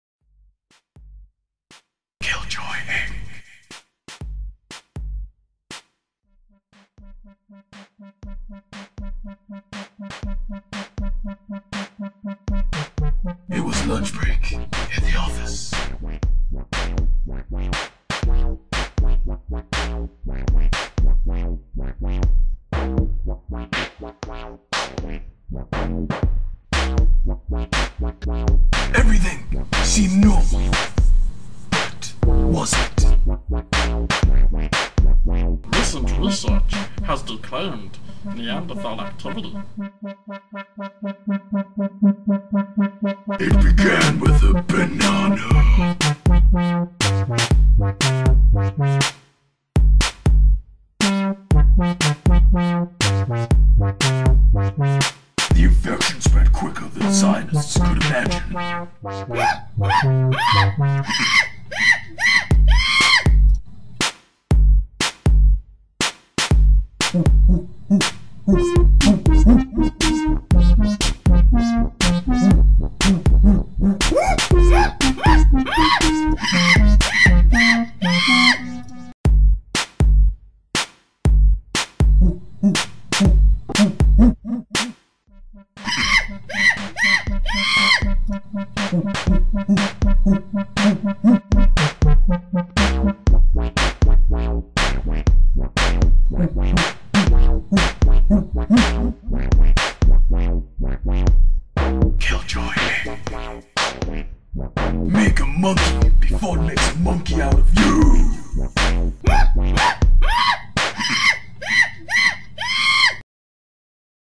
'PRI-MATE" phone app advert 2012 by the 'KILL JOY INC' boys..
Advert we made at college. Its an app for our business 'Kill Joy Inc'
the app makes a monkey outta ya face! as I go ape shizel...and no it doesn't exist yet :s

android and iphone app 'pri-mates'
recorded at gateshead college
Tyne & Wear
after effects and photoshop
comedy by the kill joy inc crew